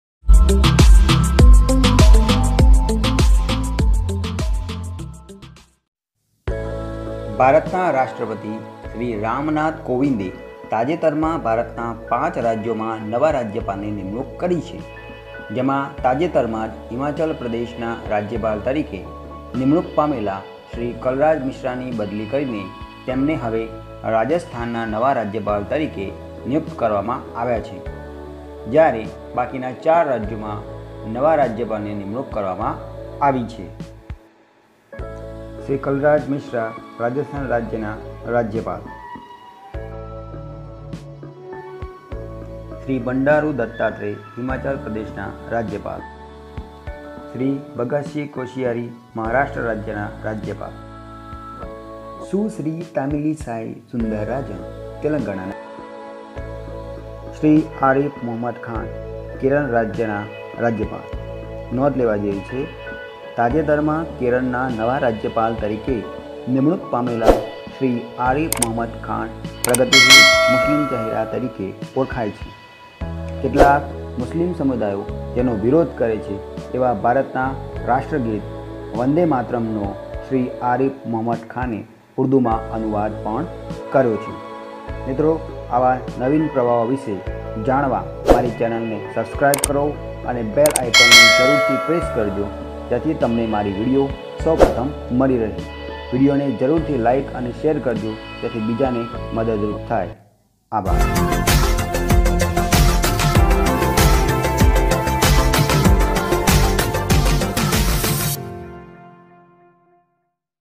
Appoint new governor in five states of India(ભારતના પાંચ રાજ્યના નવા રાજ્યપાલ)
Release - In September 2019, a new governor was appointed in five states. Bjps Tamil Nadu Head Dr. Tamilisai Soundarajan Former Governor of Dattatreya is on the list of five new governors announced today by President Ram Nath Kowind. Bandar Datatreya was appointed to replace Himachal Pradesh and Kalraj Mishra.

in this video, you will learn about the difference between yesterday and tomorrow.